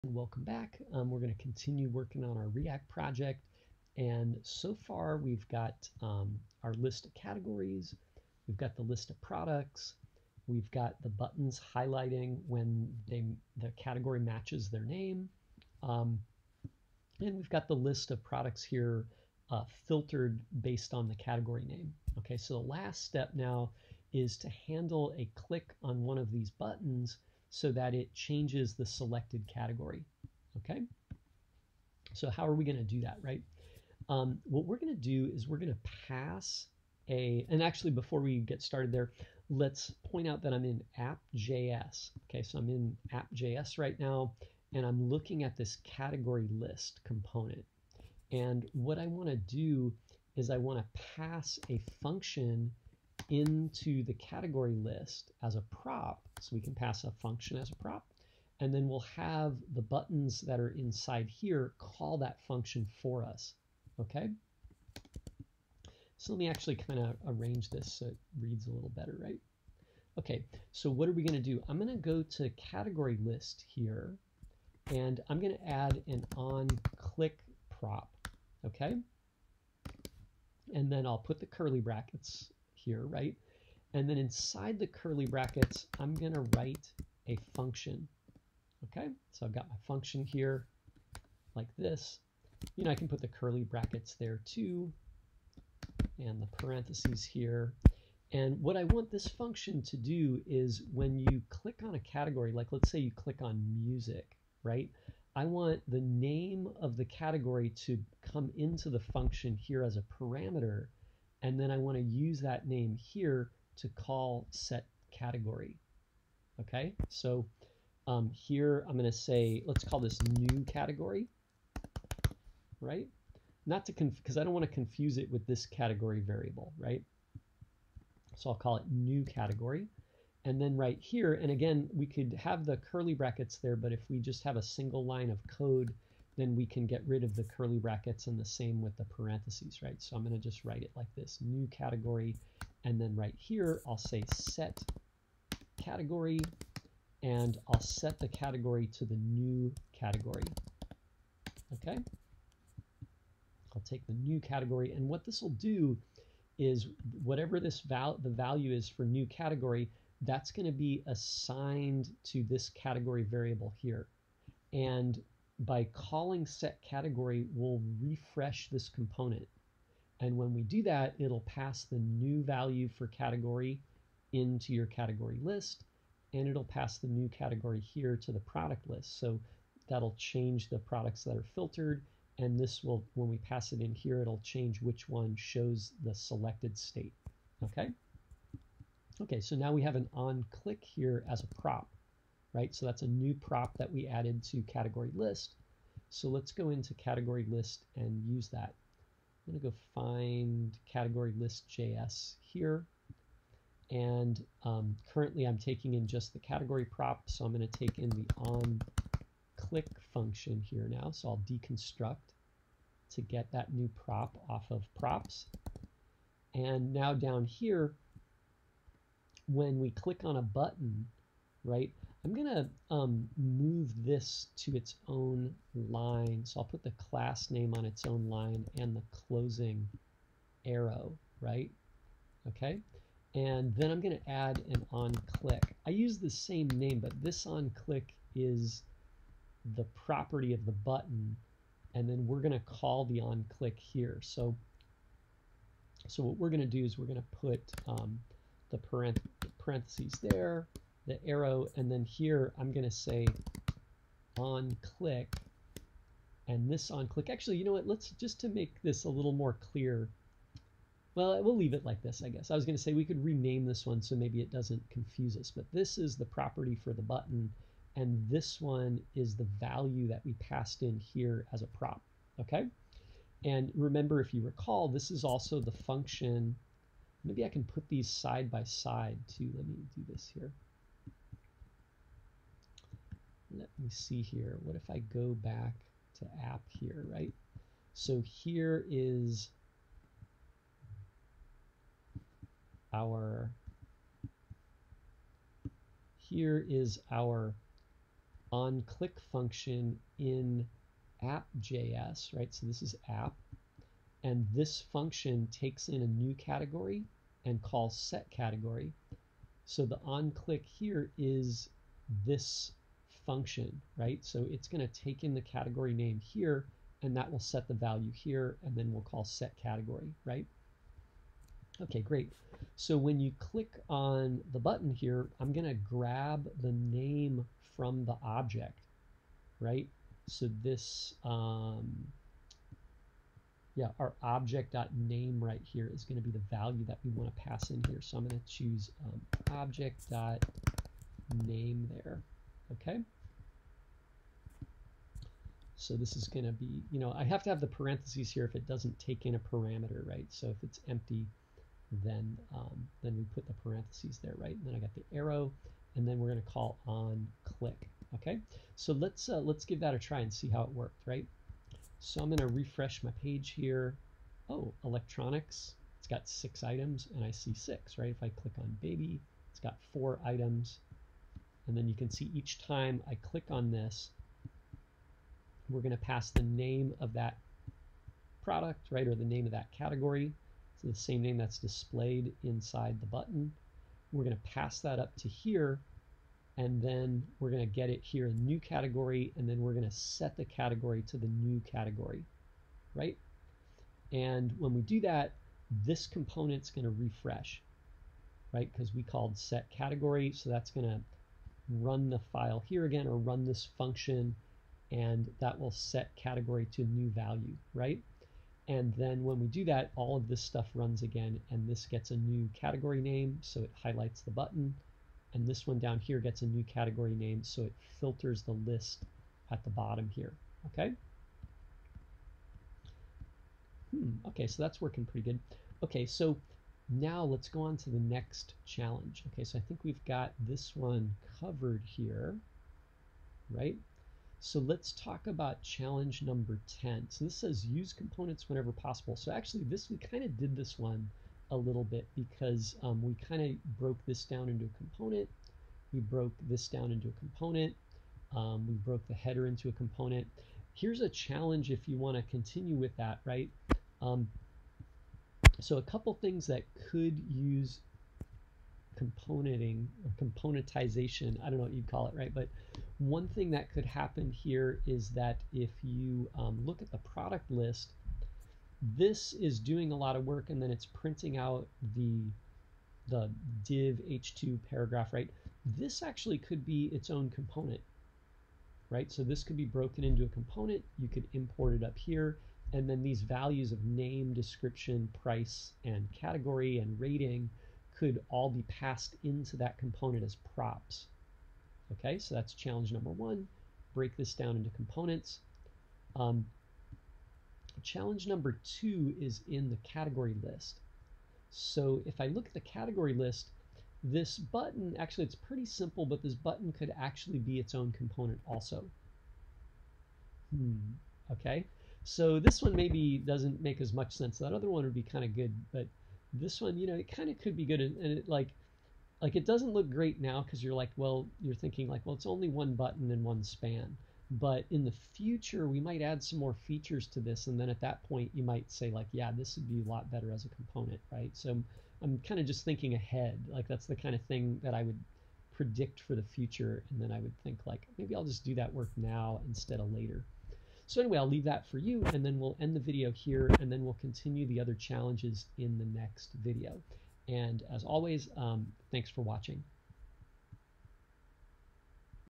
Single Page Applications - lesson 01 9
Learn react by building your single page application. This tutorial walks through the first assignment and challenges.